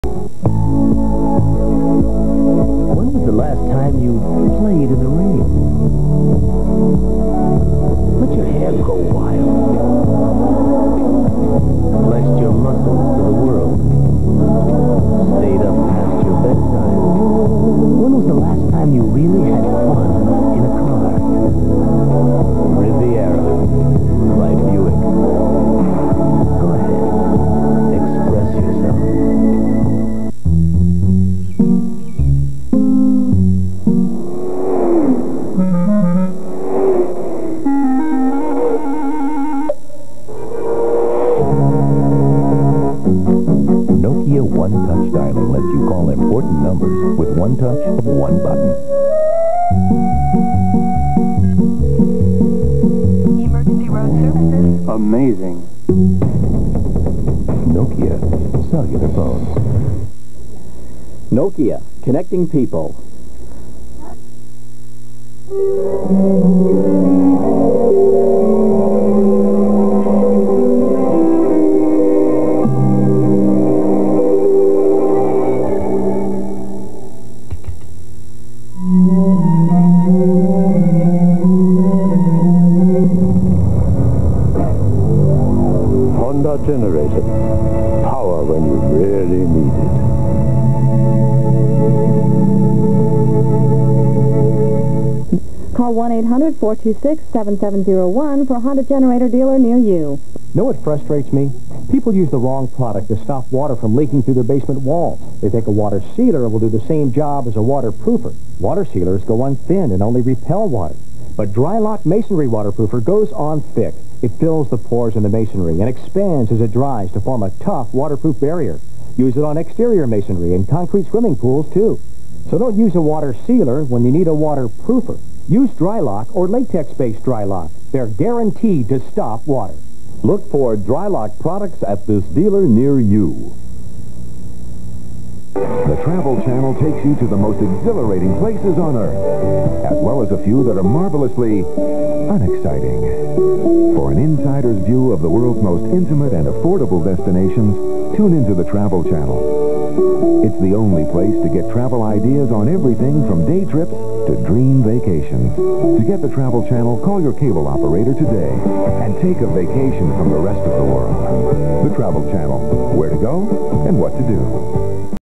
1994 Commercials
1994 Buick Riviera
Nokia Cell Phones
Honda Generators
UGL Dry-Lok Concrete Sealer
The Travel Channel